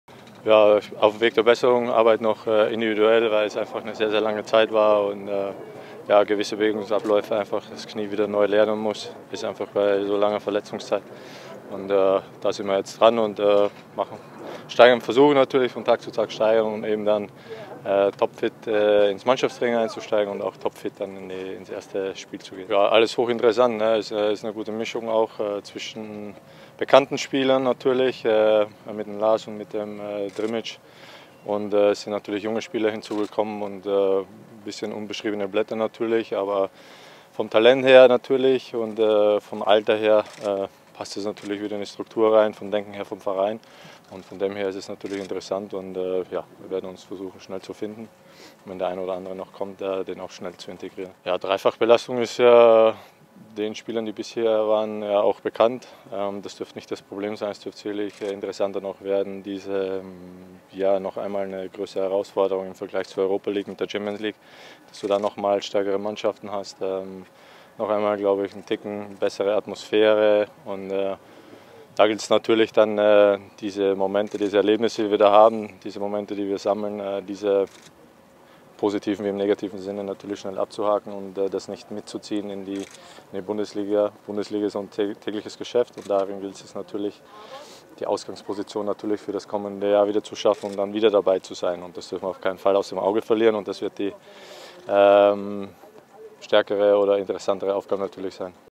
Martin Stranzl warnt vor Champions-League-Euphorie | Borussia Mönchengladbach
Martin Stranzl ist einer der erfahrensten Spieler bei Borussia Mönchengladbach. Als alter Hase gemahnte er schon zum Trainingsauftakt eine Fokussierung auf die Bundesliga an.